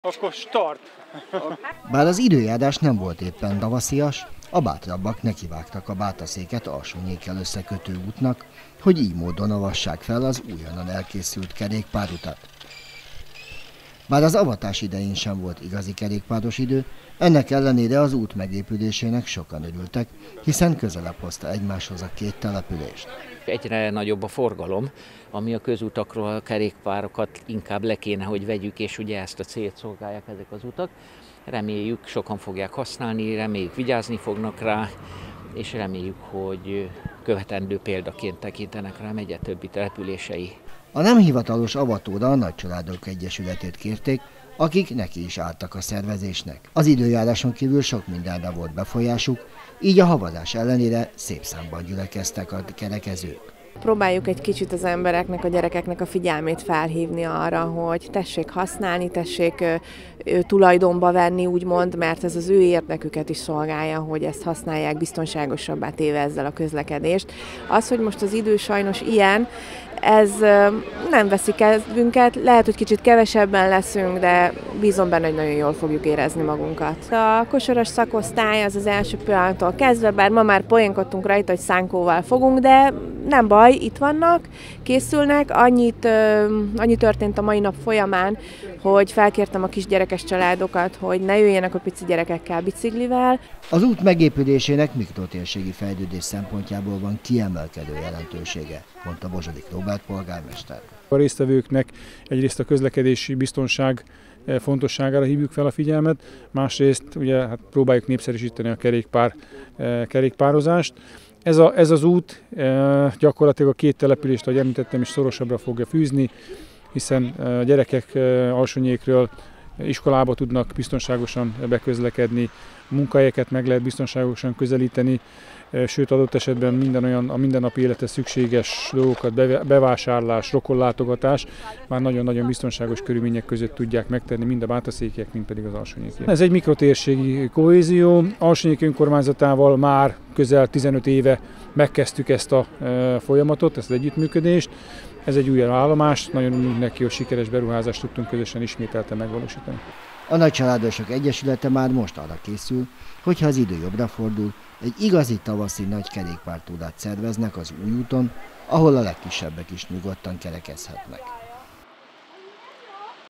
Bátaszék és Alsónyék között megépült kerékpárút - hóesésben is bringáztak 20190114
Kerékpárosan szép az élet! Igaz ez akkor is, ha az időjárás sokkal inkább a szánkózásnak kedvez, mint egy kiadós bringázásnak. A Bátaszék és Alsónyék között megépült kerékpárút népszerűsítéséért felelős program szervezői úgy gondolták próbára teszik a lakosok vállalkozó kedvét.